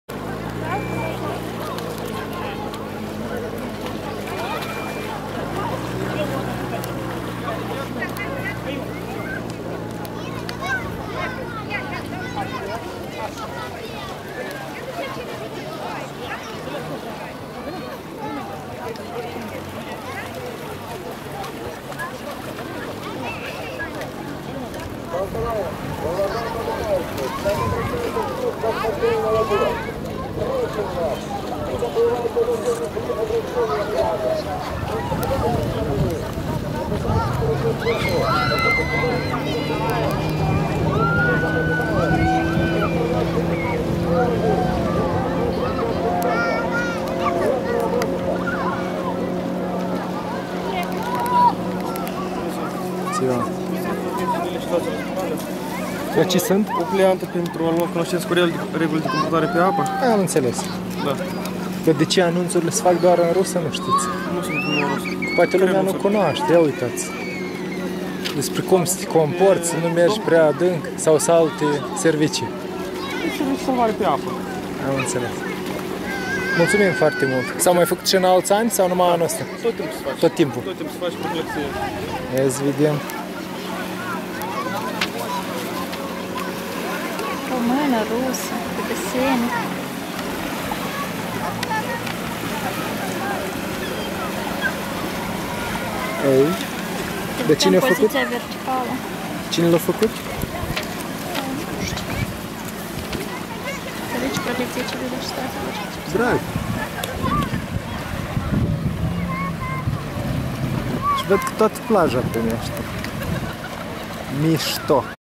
Curaj.TV // Au chemat presa să-i filmeze, au dat pliantele și au plecat
Cei de la Situații excepționale au invitat presa să filmeze cum ei distribuie pliante informaționale despre regulile de comportament în bazinele acvatice.